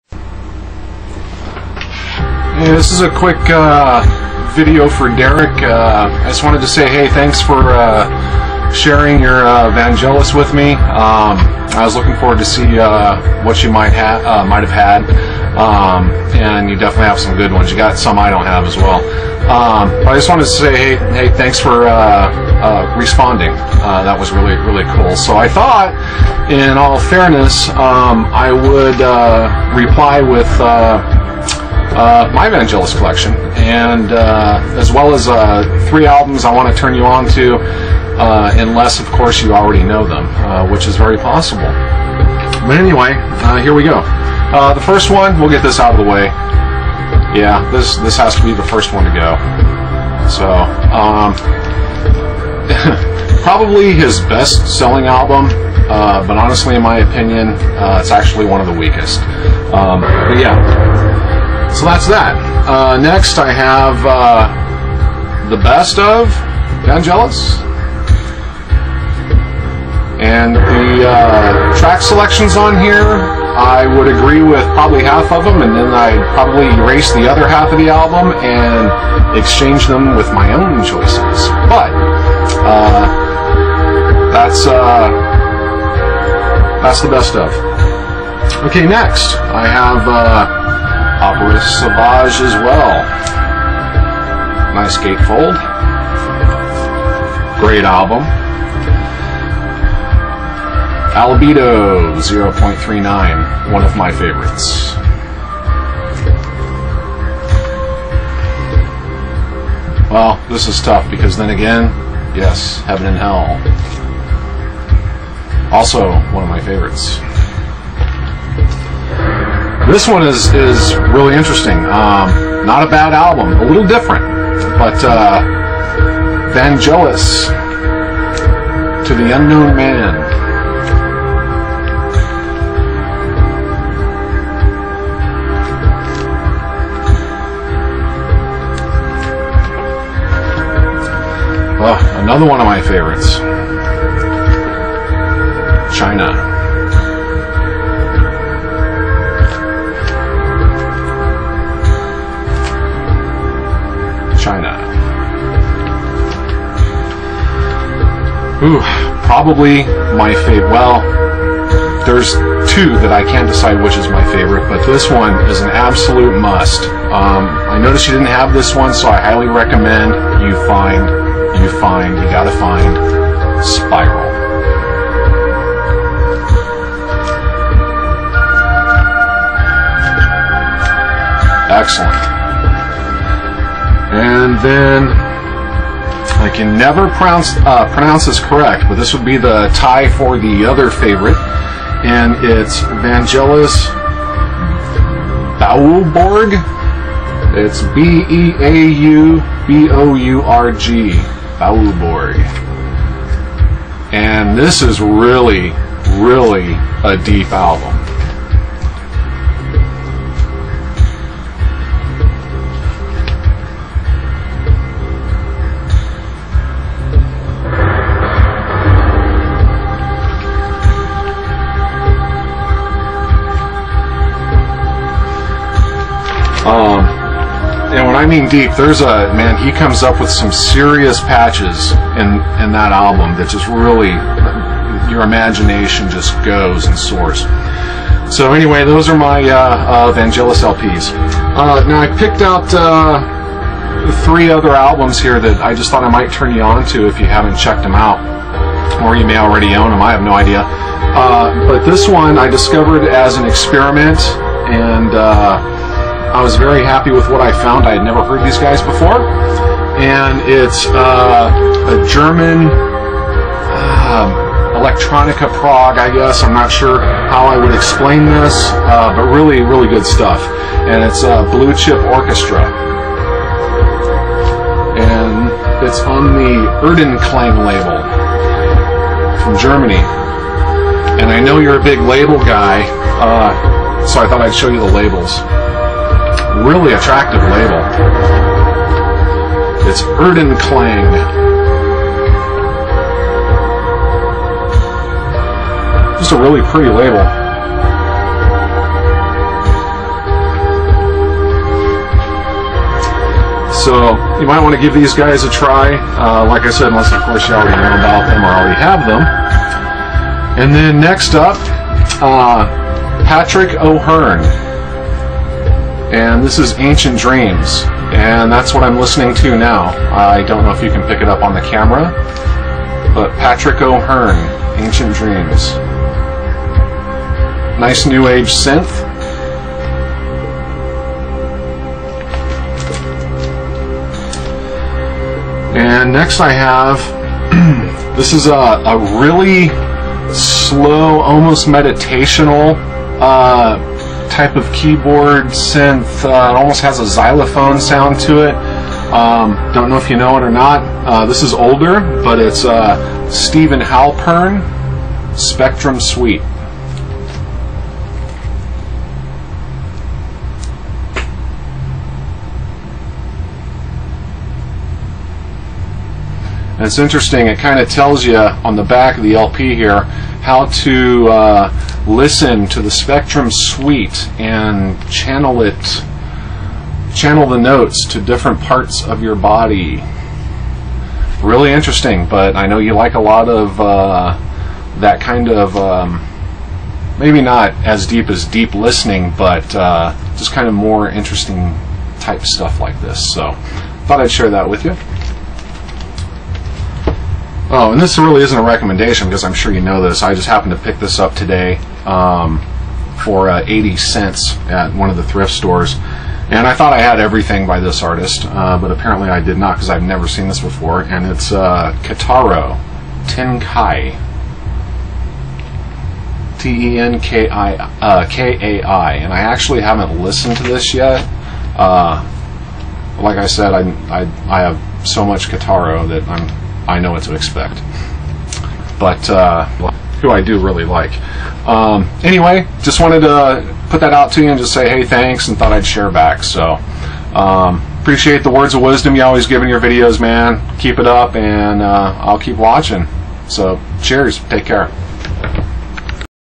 Response to Dereckvon's Vangelis Vinyl discog video and a couple recomendations
A shout out and thanks to Dereckvon for posting his Vangelis collection.